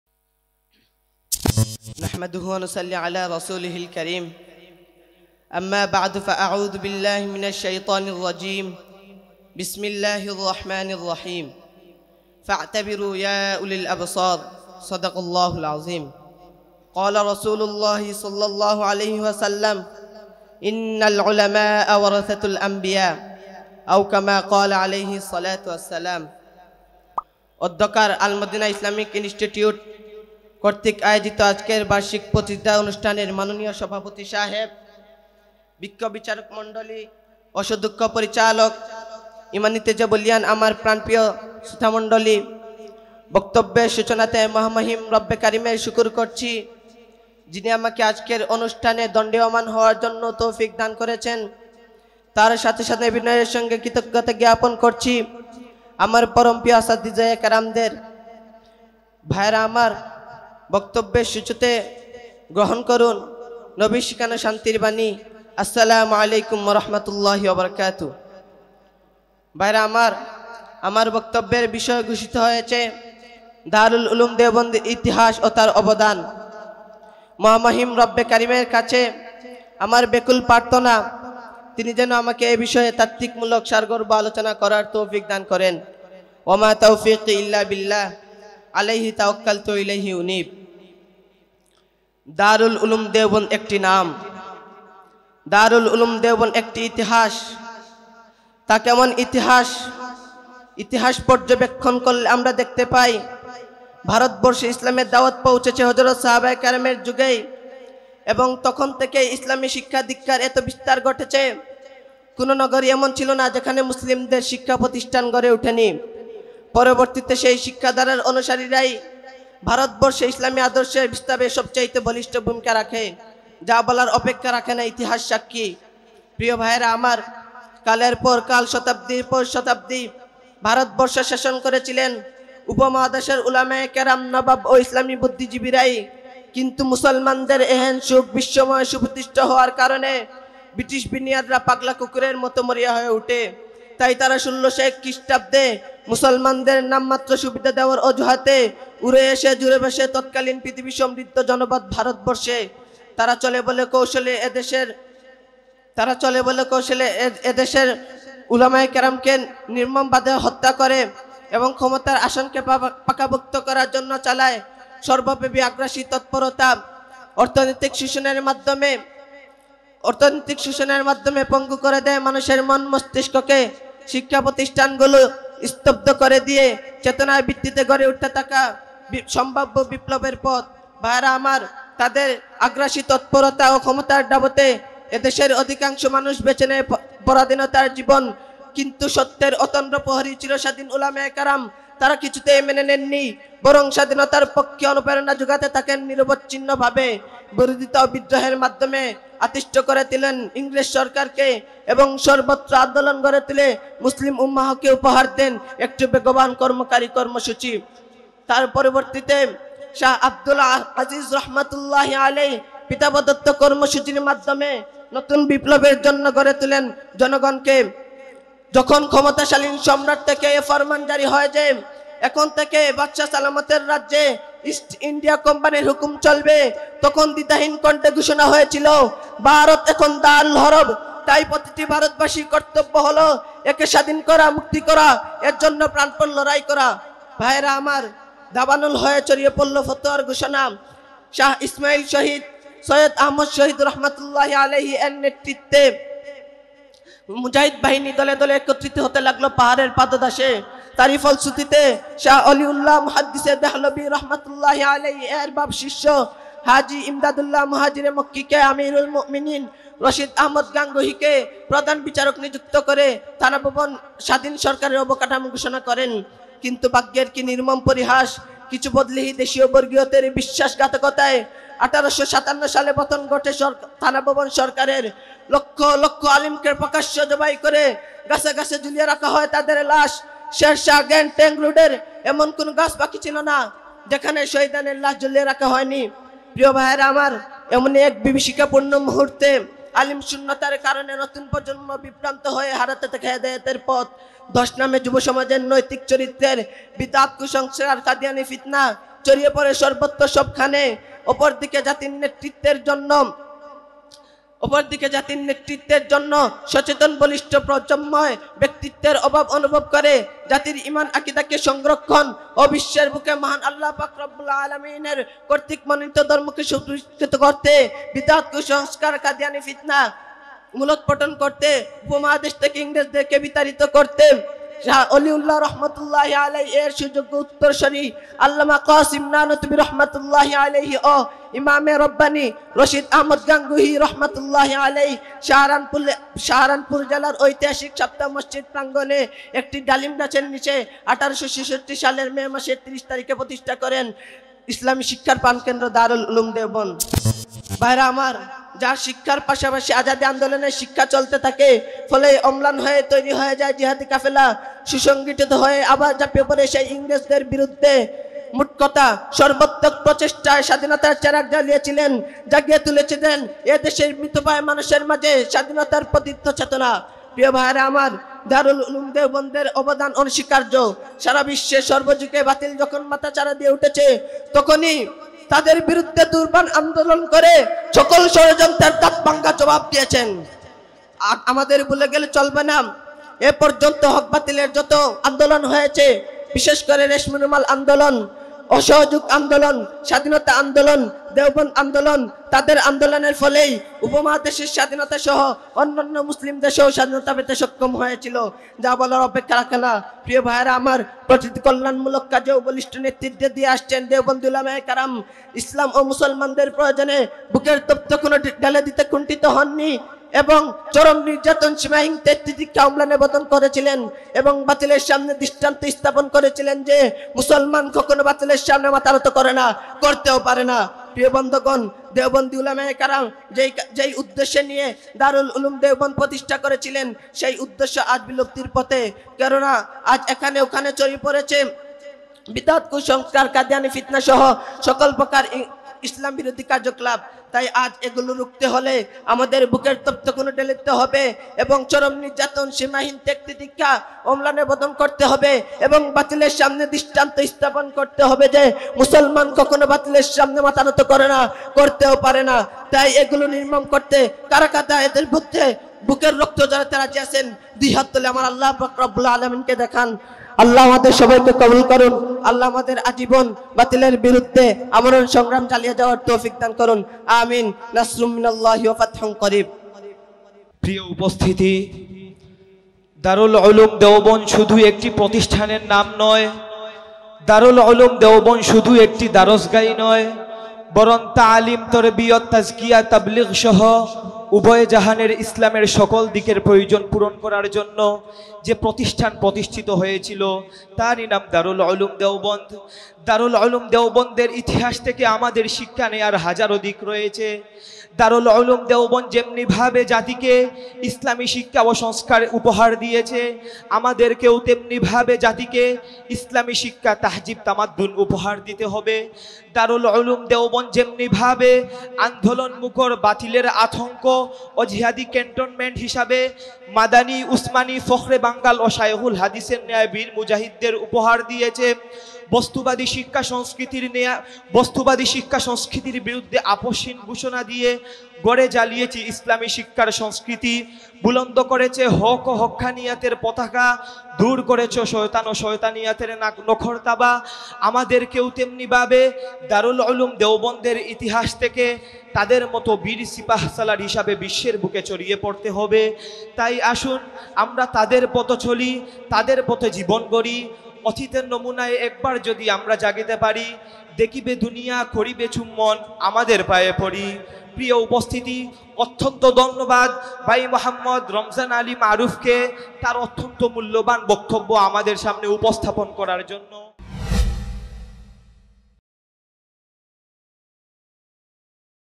07 দারুল উলূম দেওবন্দের ইতিহাস | amiisylhet | বার্ষিক বক্তৃতা প্রতিযোগিতা অনুষ্ঠান ২০২০
প্রকাশনা-০৬

বার্ষিক প্রতিযোগিতা অনুষ্ঠান-১৪৪১ হি./ ২০২০ খ্রি.
বিভাগ: বাংলা বক্তৃতা
বিষয়: দারুল উলূম দেওবন্দের ইতিহাস

আয়োজনে
আল-মাদীনা ইসলামিক ইন্সটিটিউট সিলেট
মাদানী ভিলেজ আ/এ, ১০/১, বড়শালা আবাদানি, এয়ারপোর্ট রোড

معهد المدينة الإسلامي
سلهت ، بنغلاديش
Al Madina Islamic Institute Sylhet